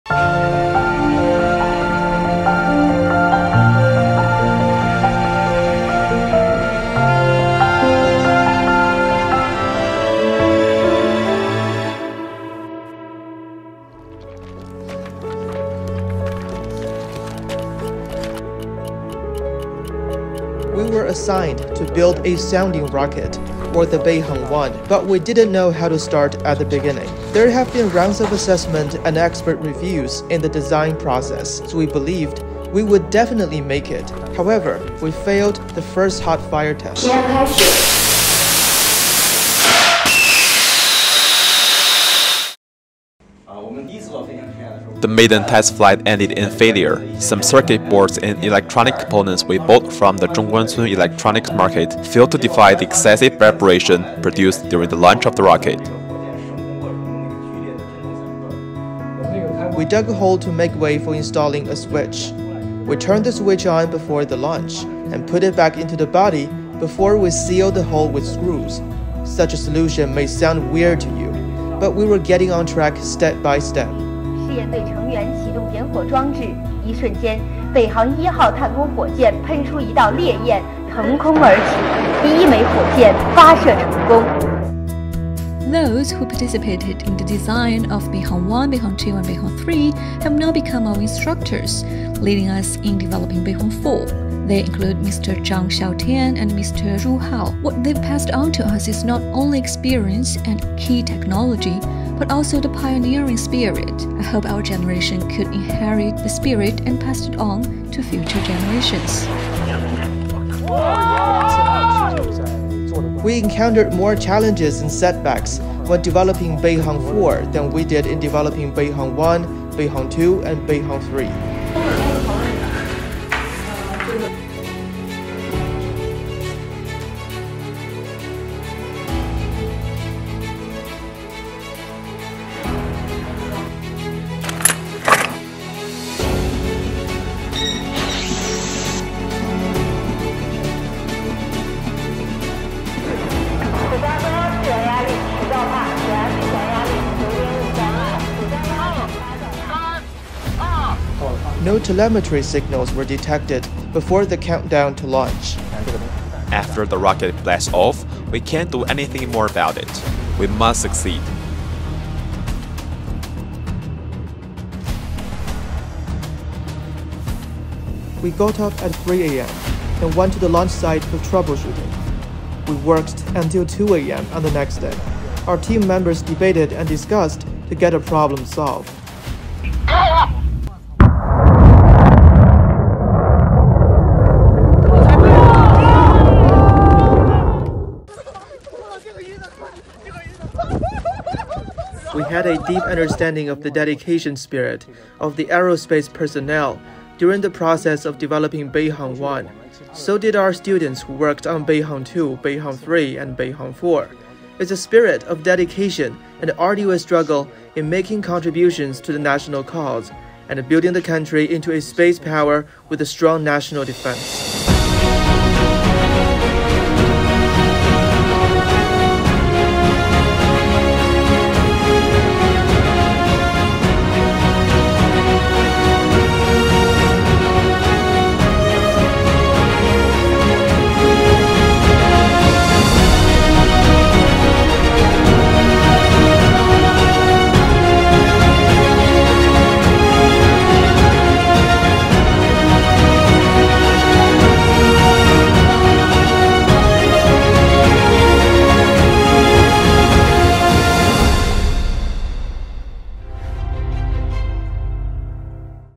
GLOBALink | Chinese space youth: We build rockets
A group of college rocket builders in China have pursued their space dream by overcoming numerous daunting obstacles